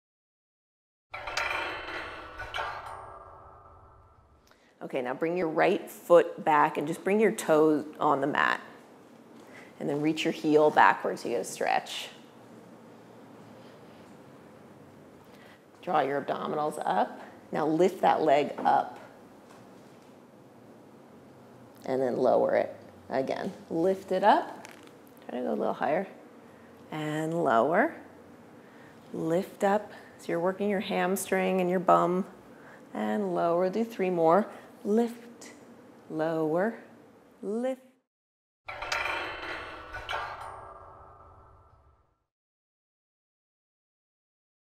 Get the Kinks Out PreNatal Mat PREVIEW
/ It only takes a few minutes to do something good for your pregnant body with Pilates. This gentle Pilates mat session is will get the kinks out while providing much needed circulation for your joints, organs, and your baby! Molly’s student Elizabeth is in her 3rd trimester so this workout is designed for any stage of pregnancy. Molly finishes with some standing arm exercises using 2 lb weights, the weights are optional you can do this series without as well. Filmed at Pilatesology Studio.